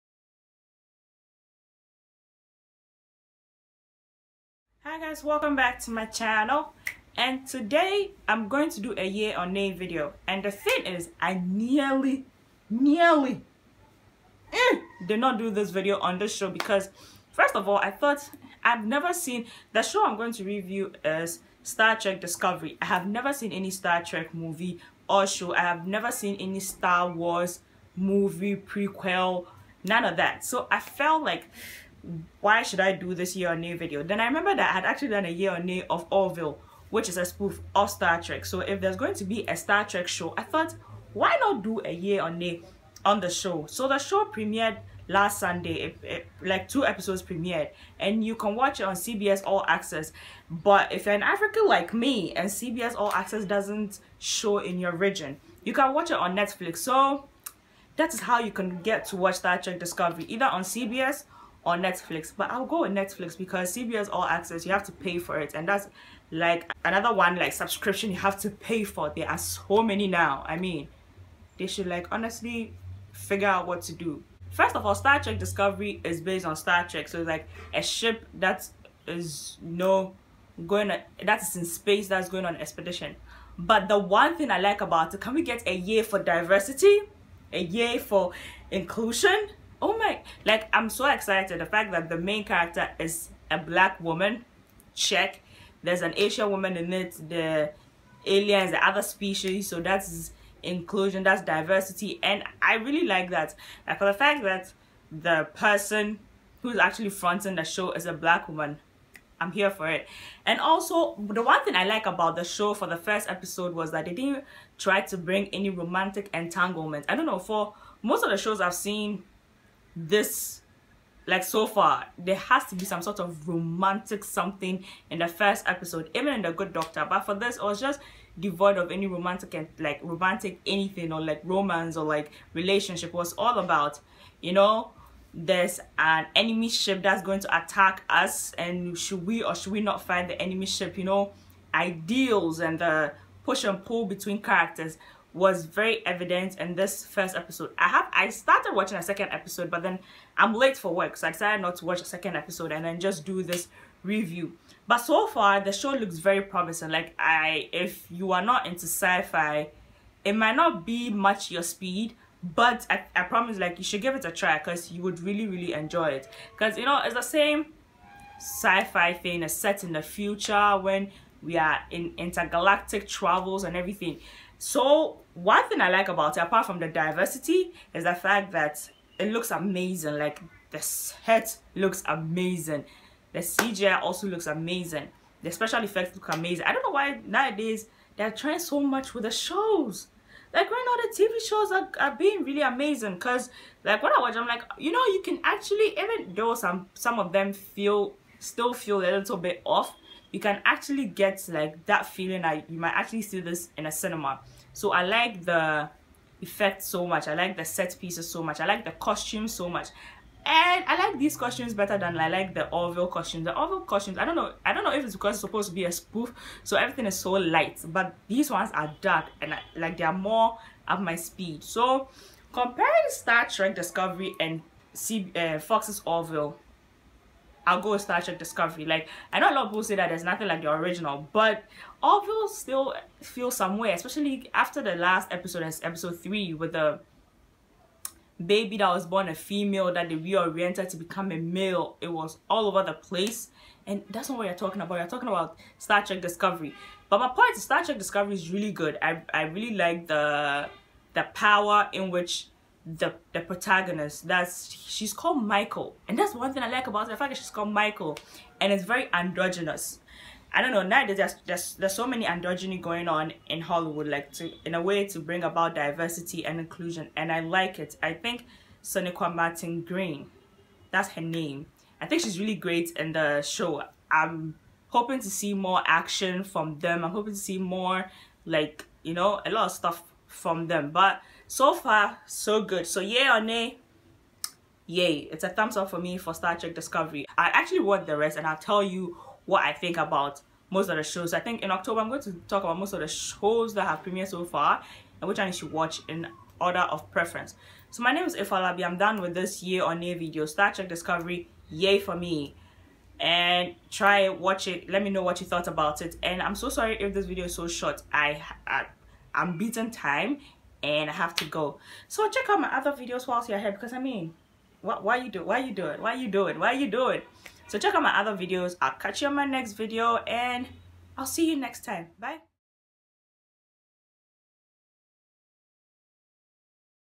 Star Trek Discovery: Yay Or Nay
I know sci-fi afficiondos will come for my neck and to my credit I have never seen any of the Star Trek movies or shows. However, this review is solely based on what I have seen so far on this show.
Hope you like it and leave your comments about the show.
(Sorry about the late upload; work has been kicking my ass)